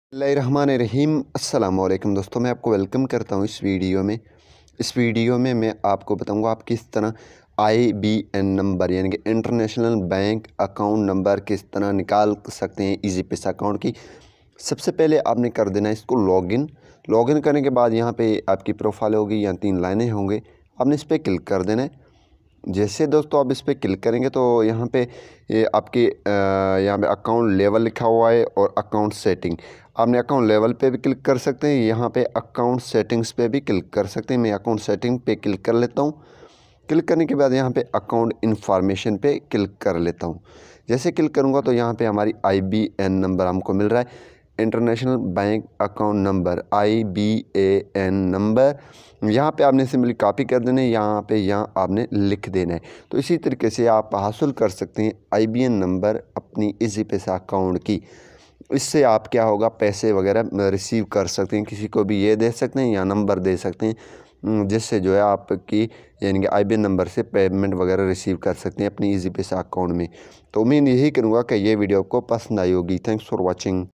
How to check easypaisa IBAN number | Easypaisa account ka IBAN number kaise nikale
Hi viewer's In this video i will tell how to check easypaisa iban number mean how to find easypaisa iban number on easypaisa account with help of app in mobile so here you learn about easypaisa iban number how to copy In easypaisa app IBAN number is auto generated. when you get easypaisa iban you can easily receive money from others.

easypaisa account ka iban number kaise nikale Yahan pa easypaisa iban code se nahi app se nikale sekten hain. IBAN means International bank account number.